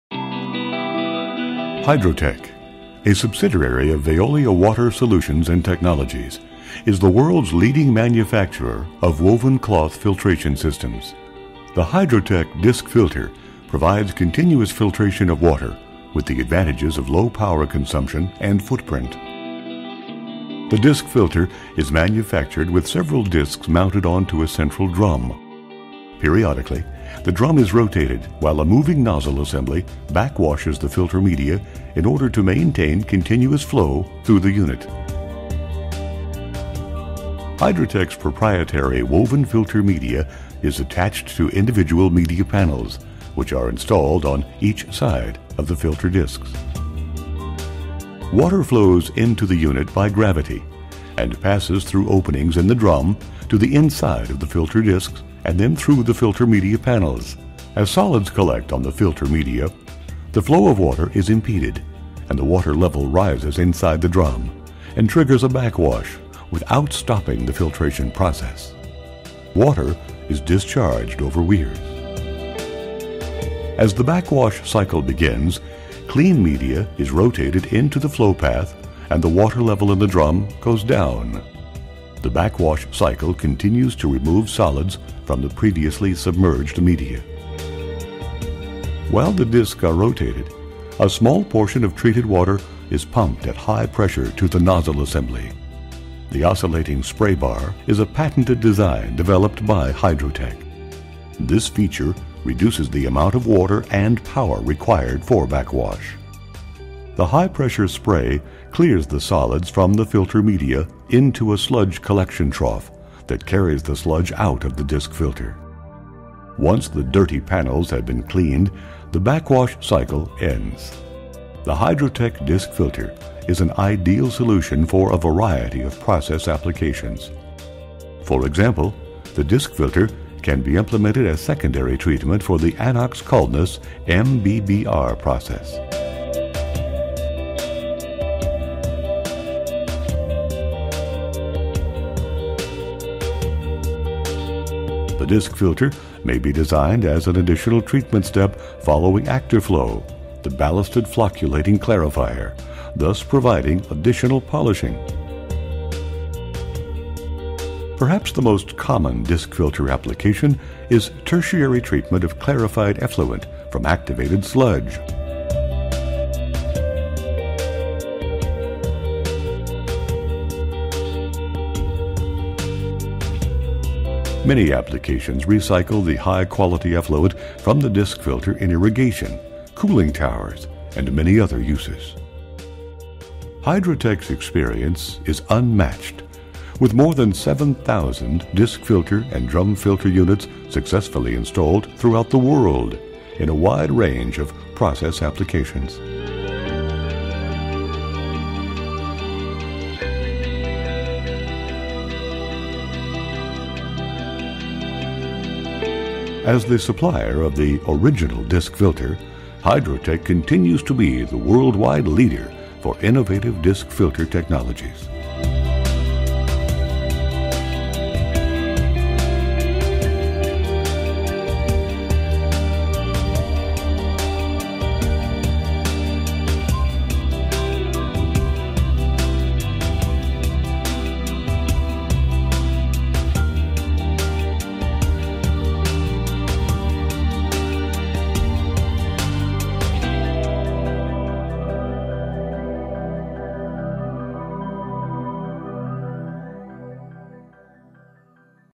Hydrotech Discfilter
Simple, effective and economical filtration in a small foot print: perfect for tertiary filtration, water reuse, and solids removal.